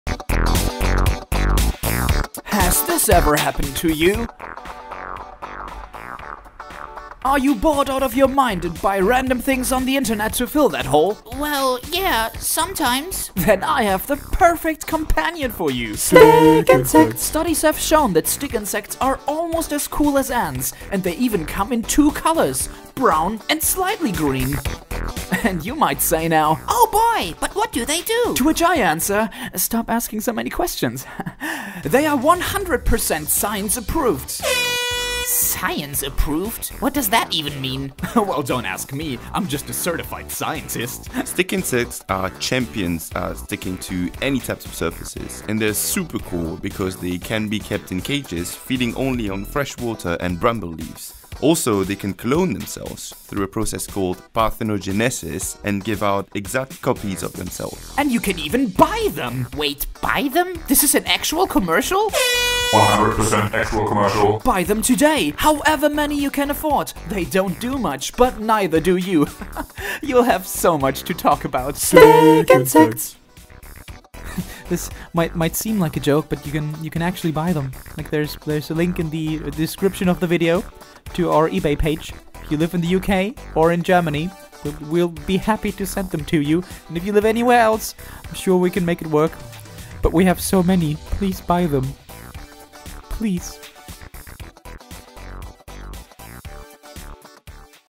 Stick Insects 100% actual commercial
STICK INSECTS! BUY THEM TOMORROW - HOWEVER MANY YOU CAN AFFORD!

This is a joke, but also kind of not, as we have so many stick insects in our lab that we really need to sell a few.

When you are watching this, they might be sold out, but we will definitely have too many again at some point.

Dr. A. Attipoe as himself
Camera, Editing, Insanity: Fabian Plum